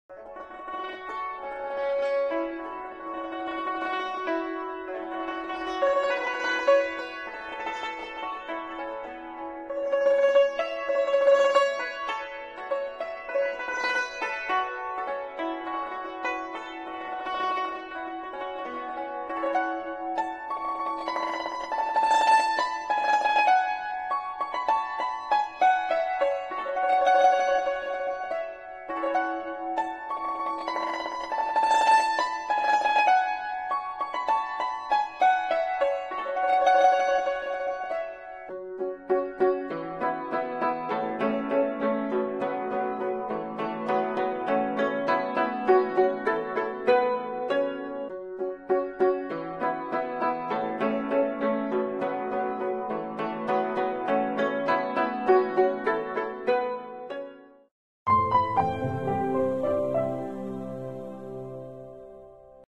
На гигантской водяной завесе воспроизвелись чудесные мифы, такие как Хоу И, снимающий солнце
Масштабное живое мультимедийное шоу фонтанов «Богиня Цзинъюэ» на днях было показано в государственном лесном парке Цзинъюэтань в городе Чанчунь провинции Цзилинь, Китай. На гигантской водяной завесе с помощью музыки, лазеров, 3D-изображений и других технологий воспроизводится мифическая история происхождения богини Цзинъюэ, Стрельбы Хоу И в солнце и другие захватывающие сцены. Сцена была освещена, привлекая большое количество туристов для просмотра.